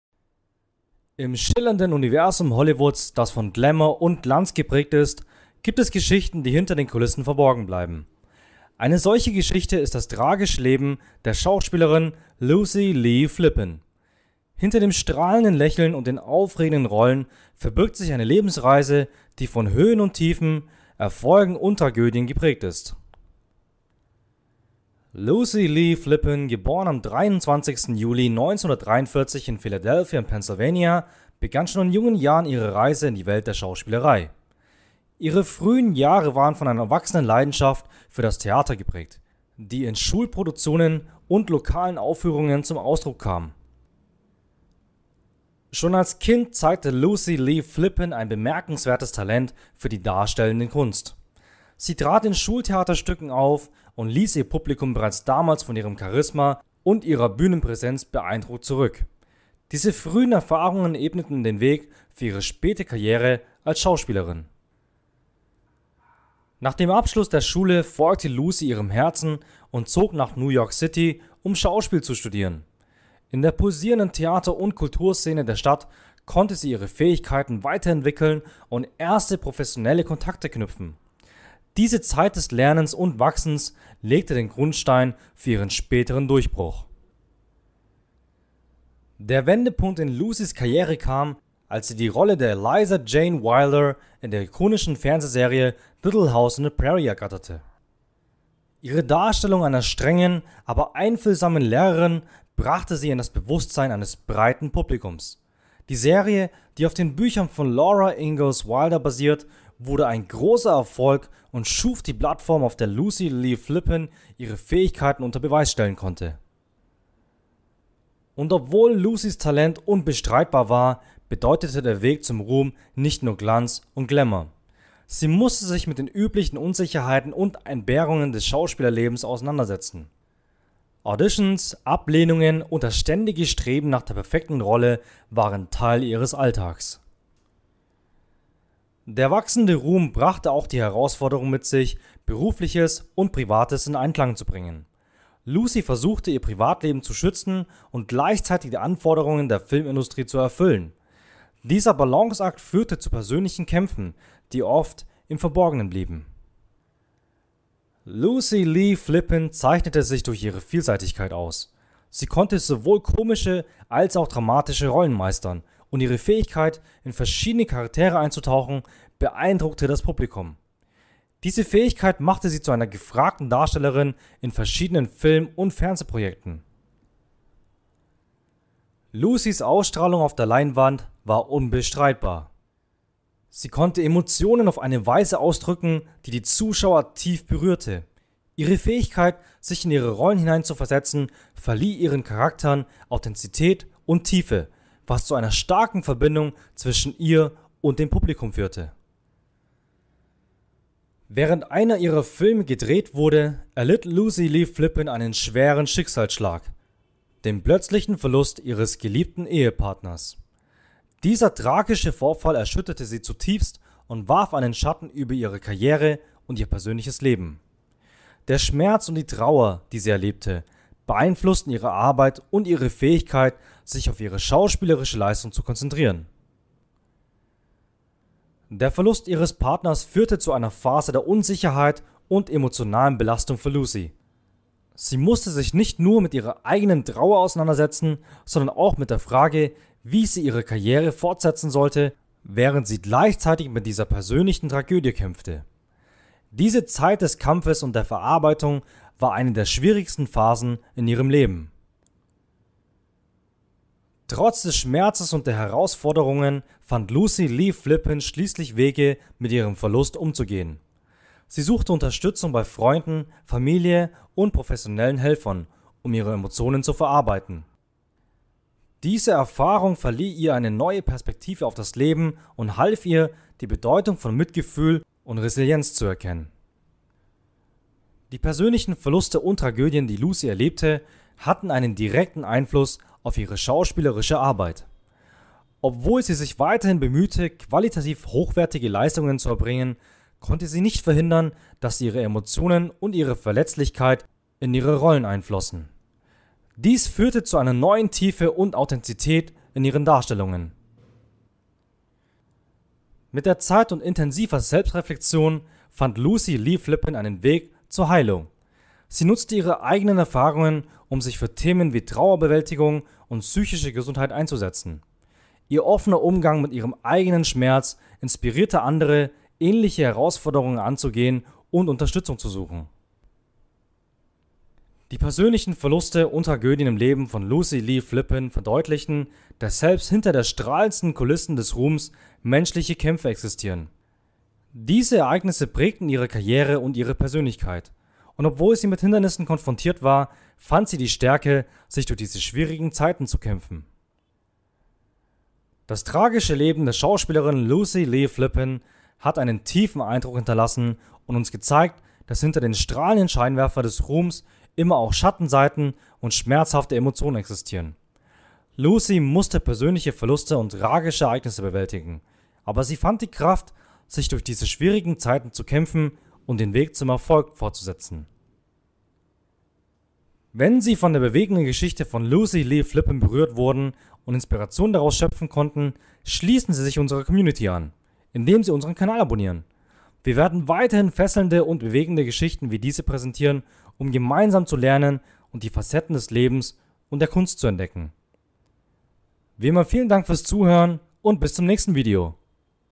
Das Leben und das traurige Lucy Lee Flippin - Star in Little House on the Prairie
Im schillernden Universum Hollywoods, das von Glamour und Glanz geprägt ist, gibt es Geschichten, die hinter den Kulissen verborgen bleiben. Eine solche Geschichte ist das tragische Leben der Schauspielerin Lucy Lee Flippin. Hinter dem strahlenden Lächeln und den aufregenden Rollen verbirgt sich eine Lebensreise, die von Höhen und Tiefen, Erfolgen und Tragödien geprägt ist.